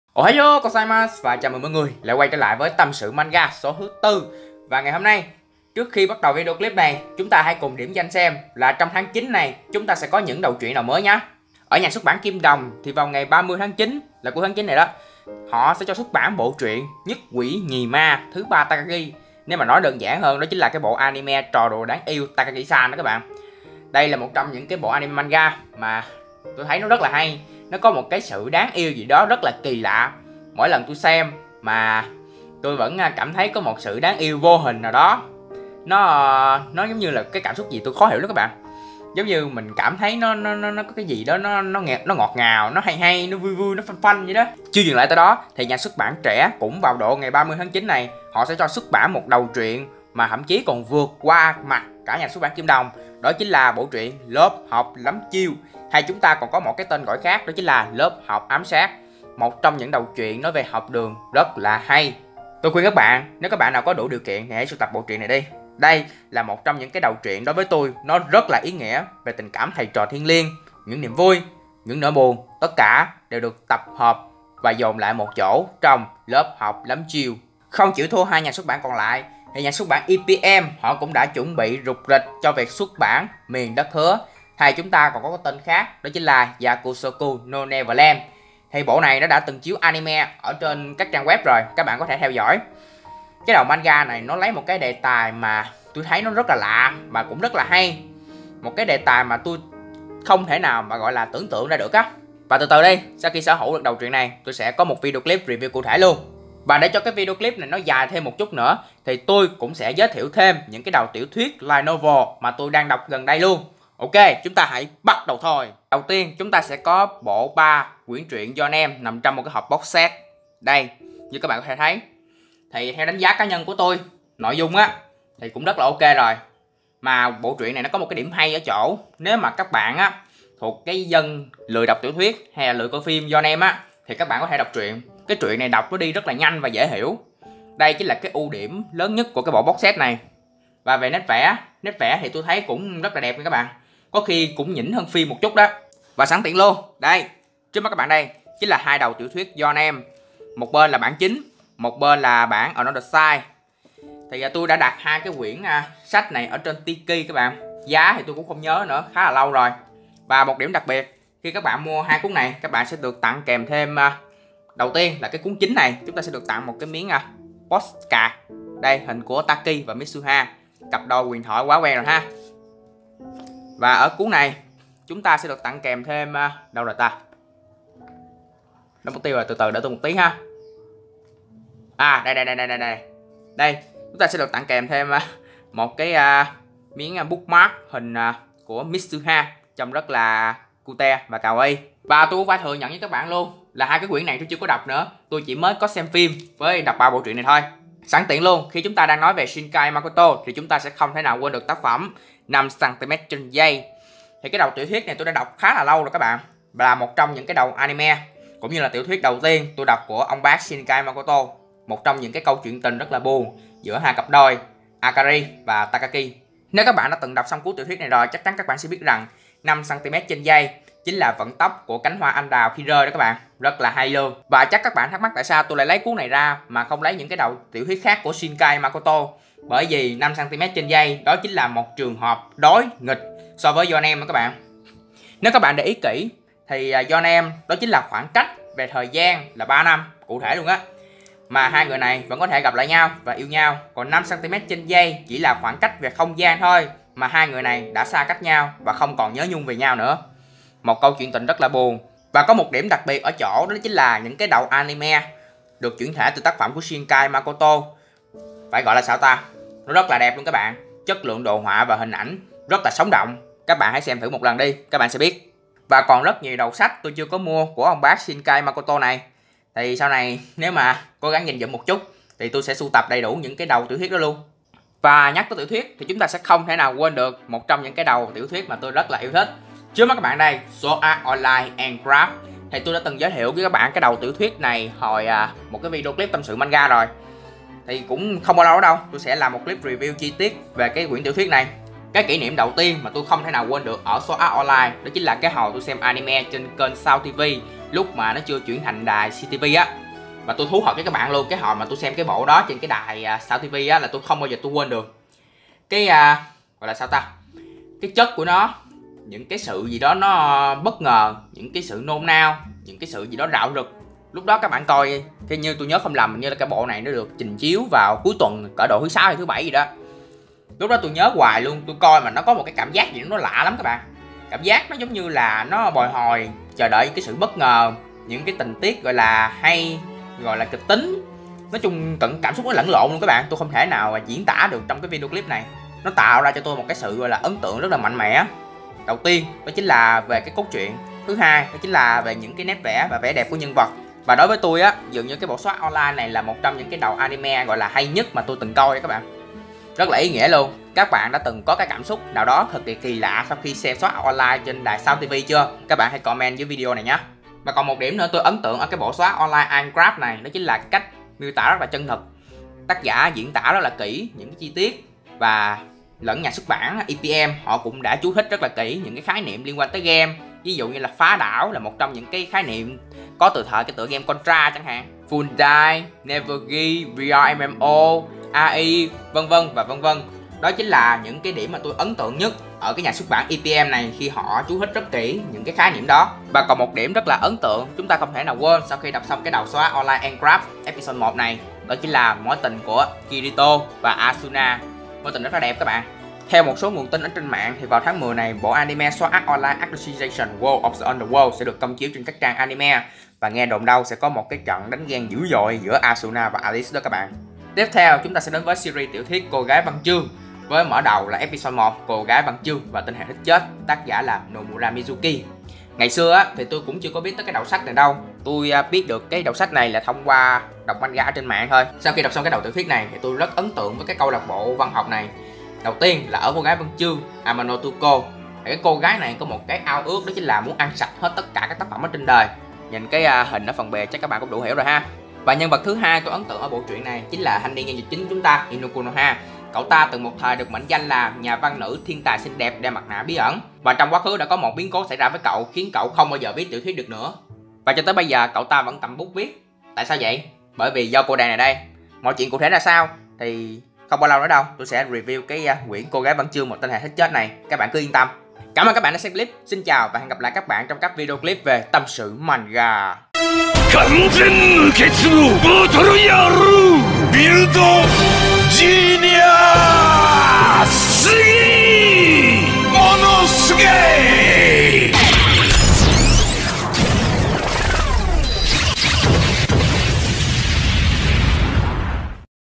Tâm Sự Manga #4: Tâm Sự Thêm Về Một Số Đầu Light Novel. Kỉ Niệm Lần Đầu Xem SAO
- Tâm sự thêm về một số đầu Light Novel.
- Trải nghiệm lần đầu xem Sword Art Online.